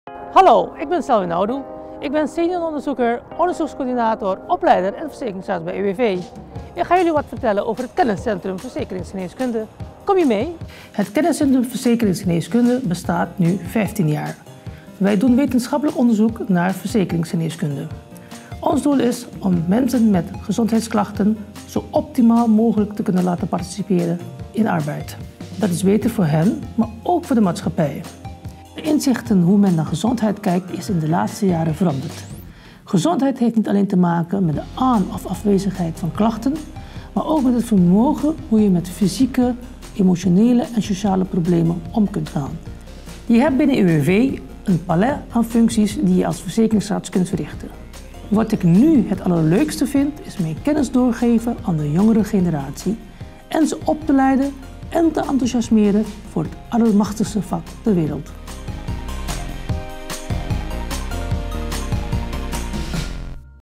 Senior onderzoeker Selwin - Teaser
Selwin is senior onderzoeker, onderzoekscoördinator, opleider en verzekeringsarts bij UWV en vertelt over het kenniscentrum Verzekeringsgeneeskunde van UWV.